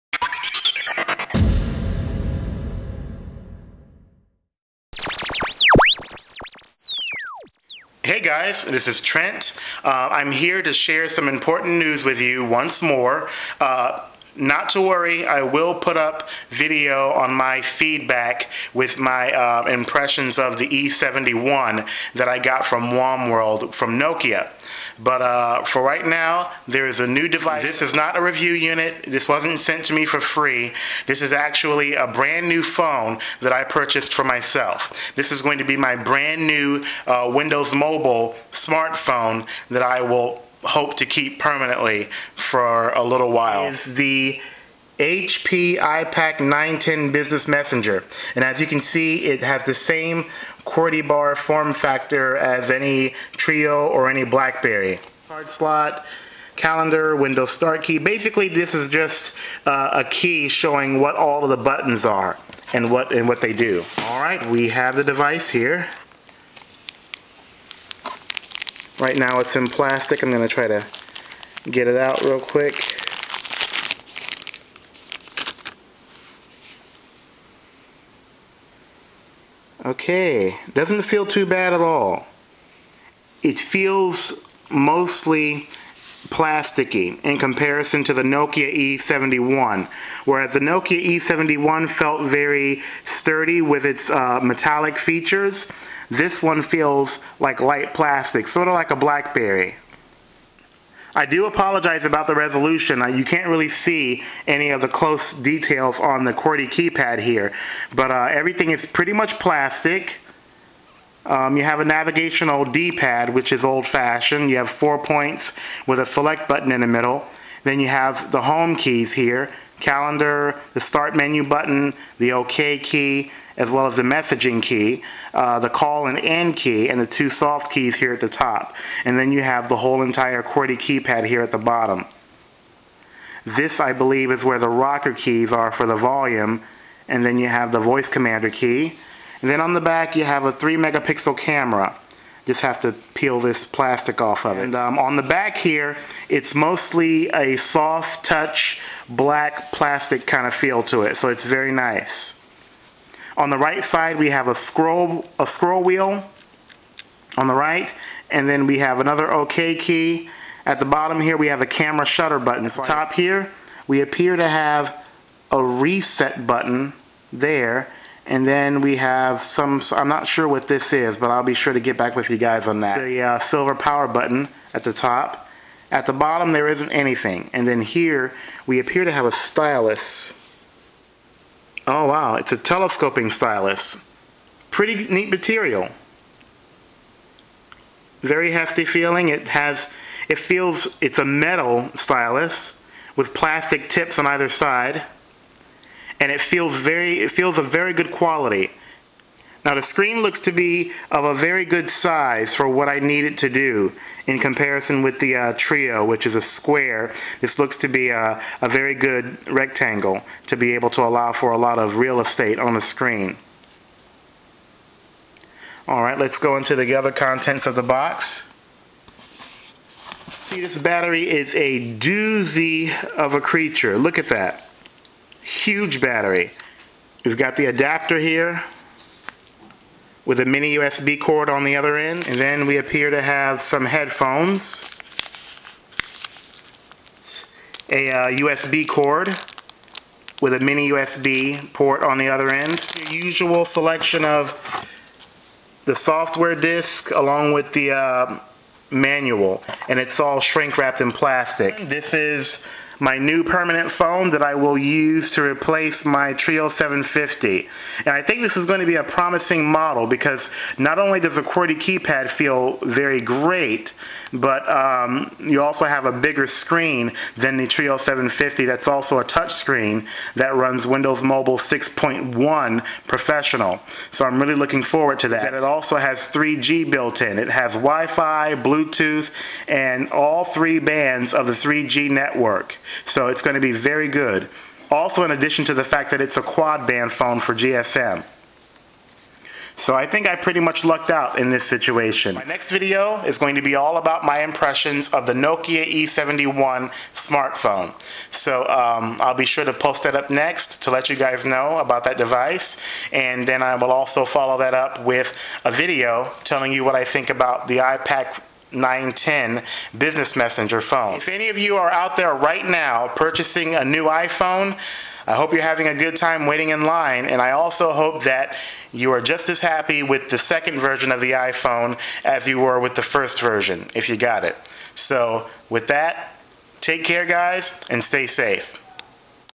HP 910 Business Messenger unboxing
This is my new permanent smartphone that I have purchased for myself to replace the Treo 750.